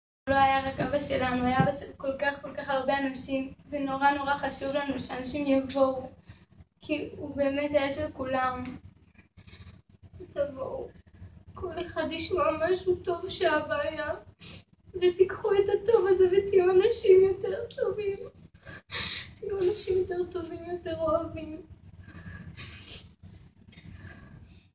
Times of Israel-Family of rabbi killed in terror attack urges public to attend funeral
Credit: Mount Hebron spokesperson